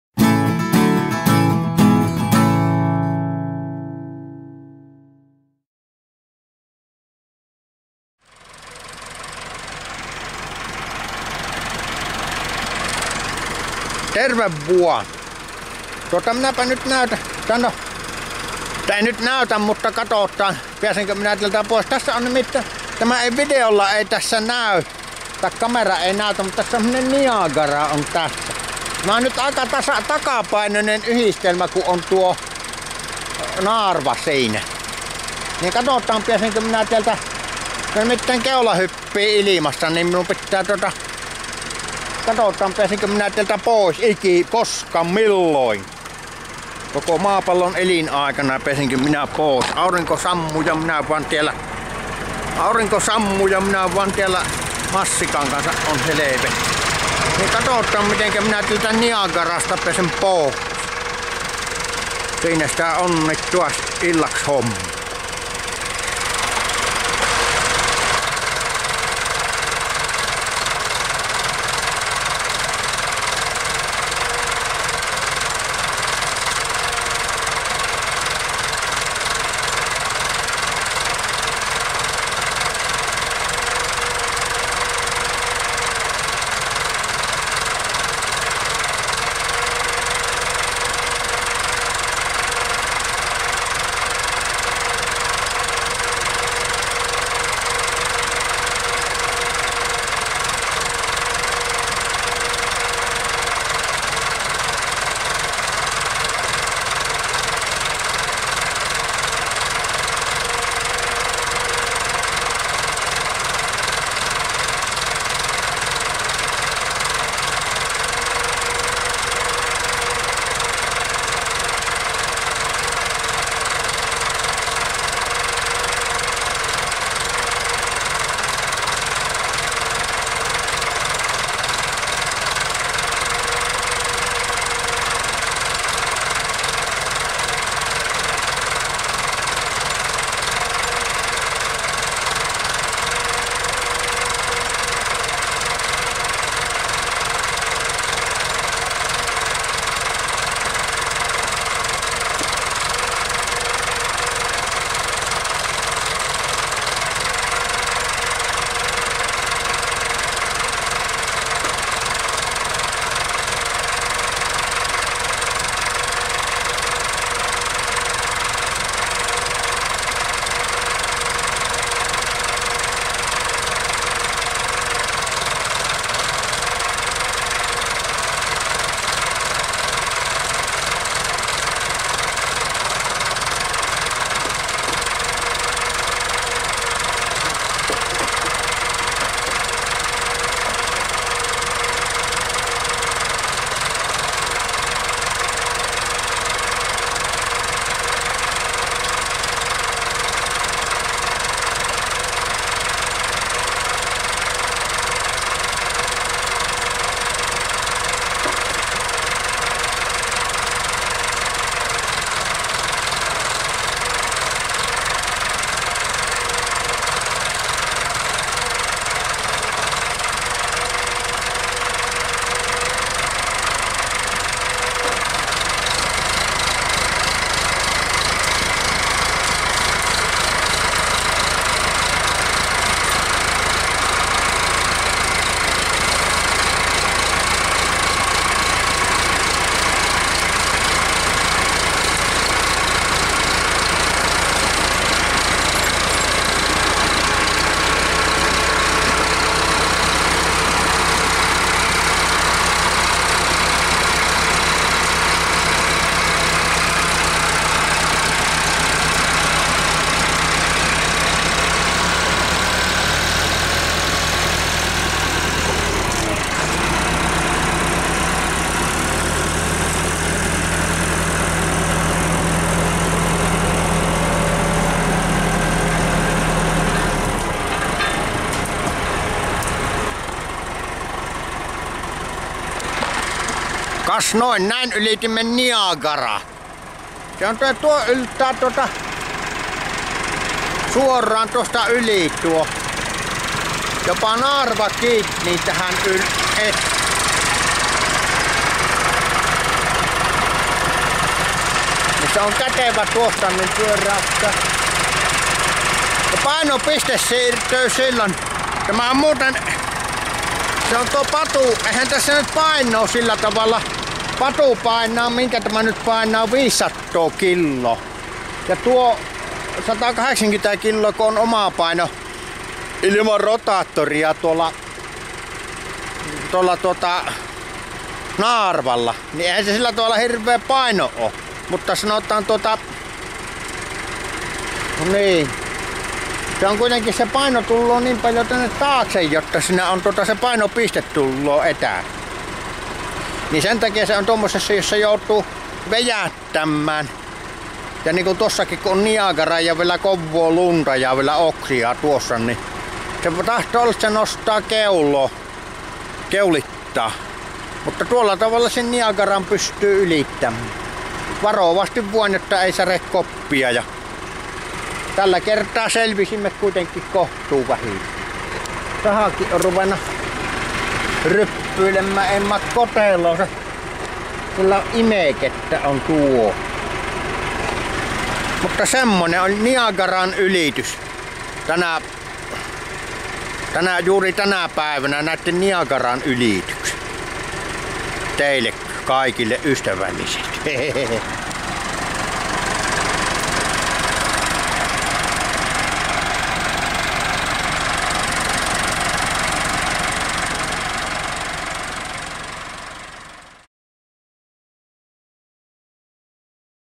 MASSIKKA ja NIAGARA
Niagaran ylitys takapainoisella  Massikalla, perässä Patruuna 50 kourakuormain, sen puomin nokassa Naarva 1000-23 ja perässä vielä Syke Patu prosessori.